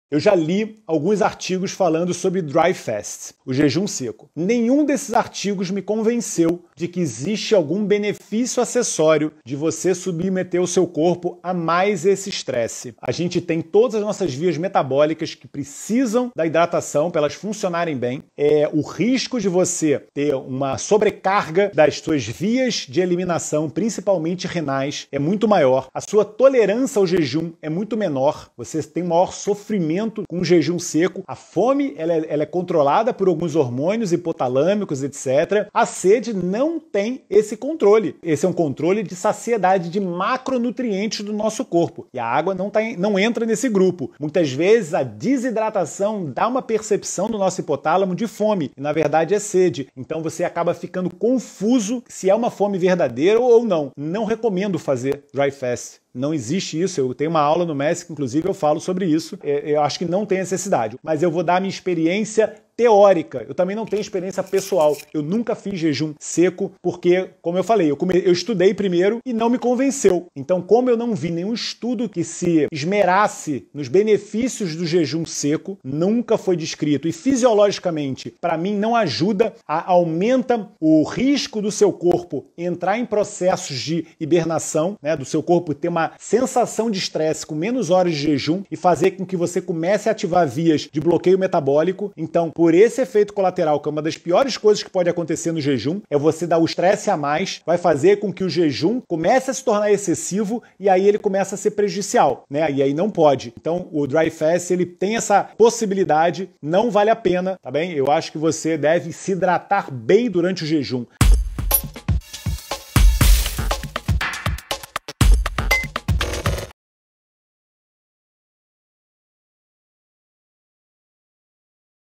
Saiba porquê que eu não recomendo dry fast.
Você acaba ficando confuso com estes tipos de sinais e comportamentos do seu corpo.